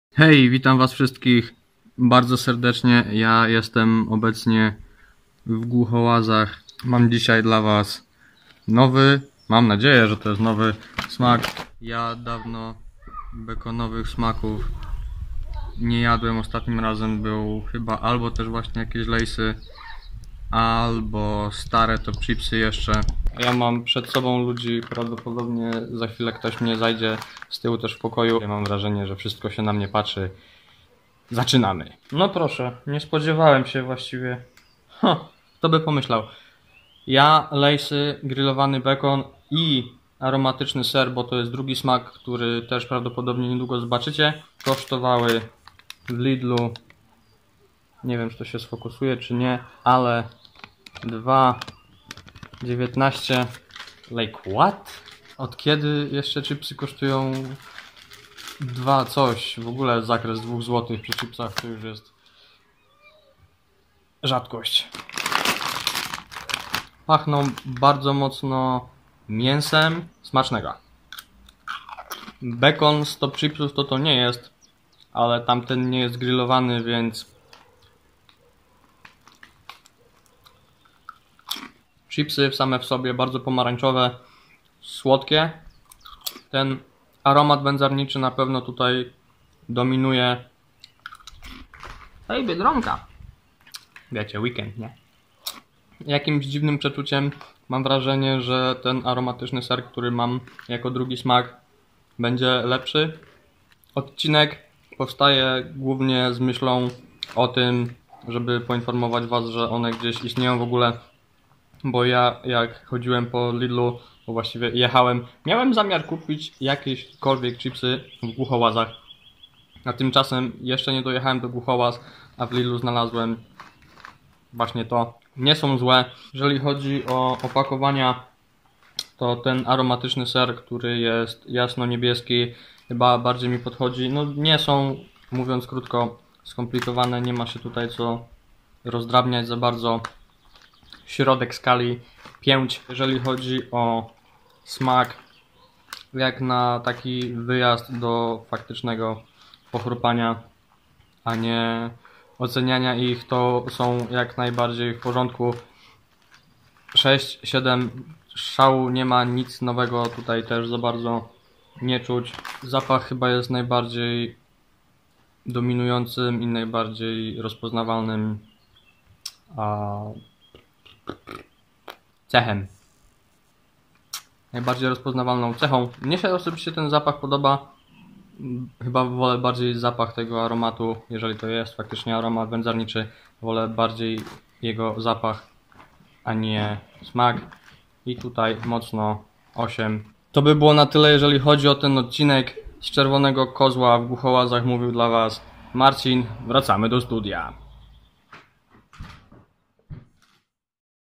Lay's Grillowany Bekon! - Daj Jednego S2E10
Hej! Weekend spędziliśmy wraz z Kasią w Głuchołazach! W międzyczasie udało mi się kupić kilka smaków ;). Oto pierwszy z nich! :)